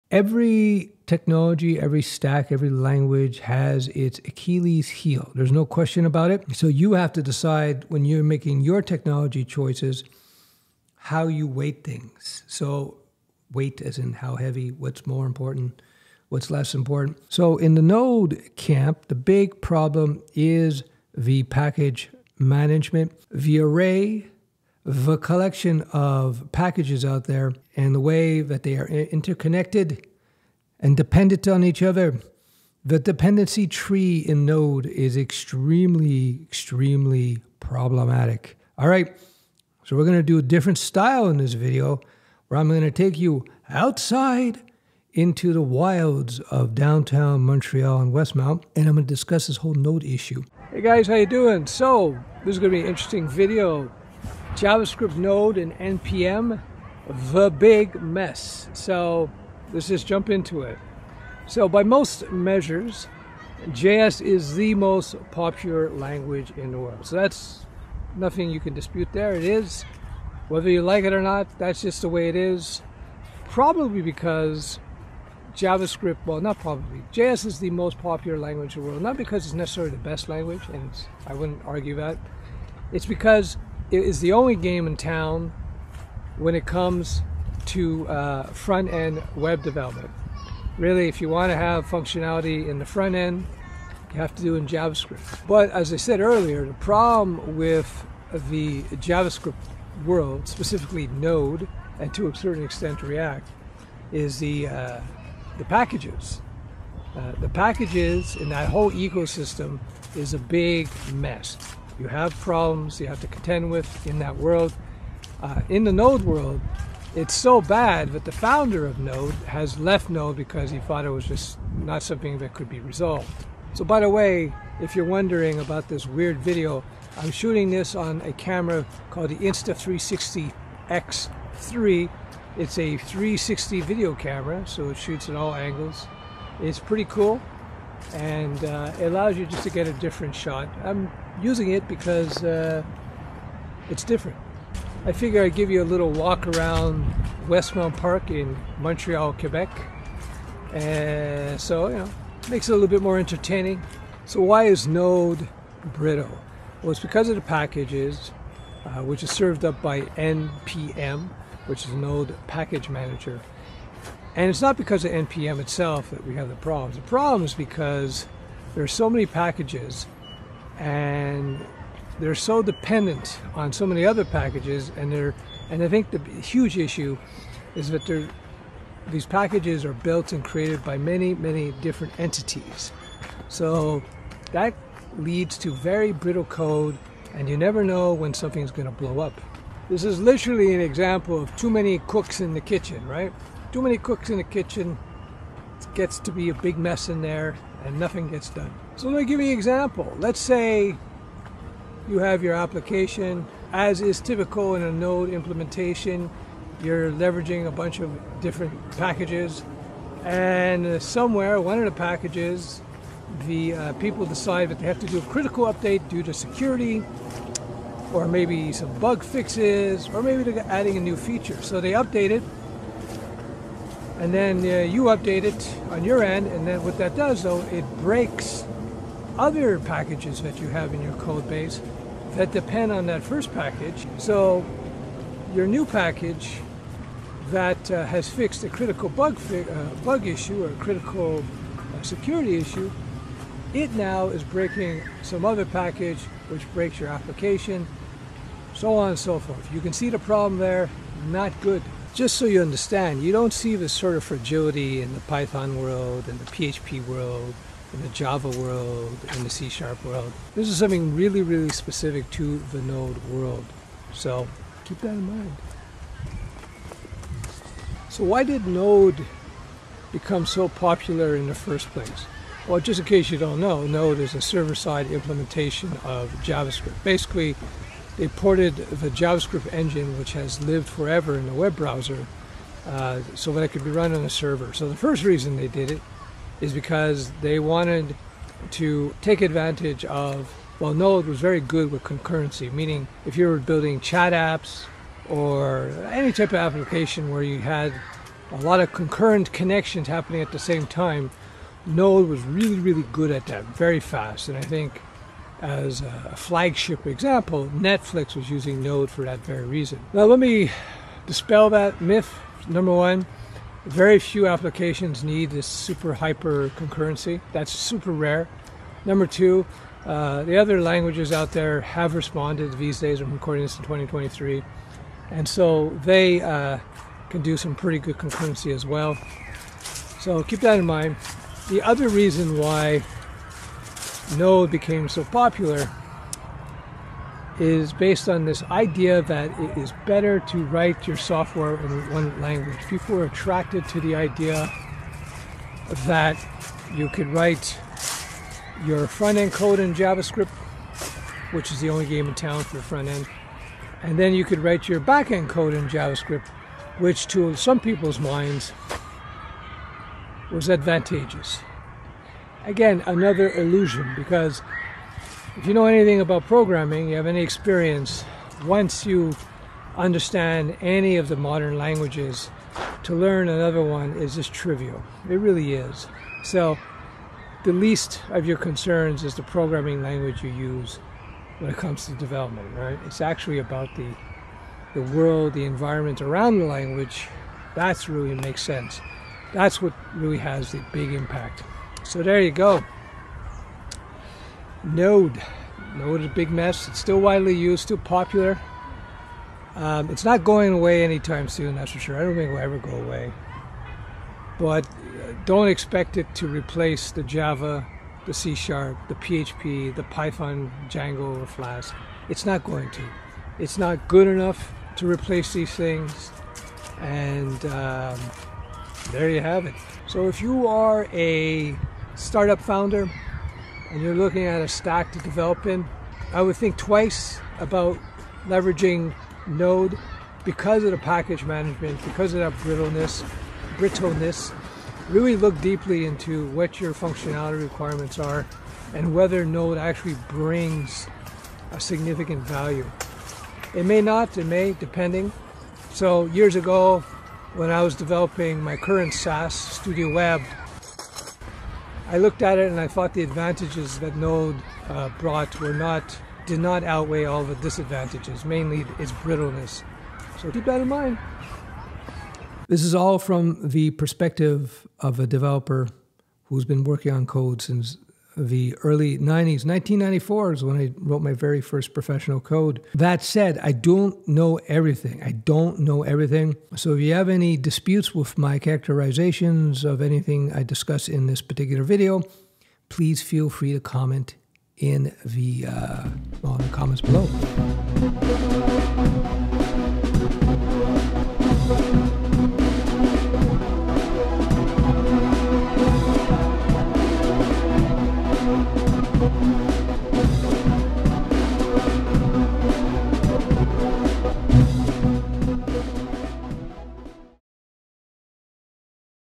Node has a HUGE Problem ...
Node.js is popular, but it has a huge problem the huge array of JavaScript libraries. In this video I give you the reason why Node.js and NPM are a mess.

The Naked Ape:

SUPPLEMENTS THAT WORK AMAZING FOR ME:

Protein Essentials Beef Gelatine Powder:

Thanks!

Stef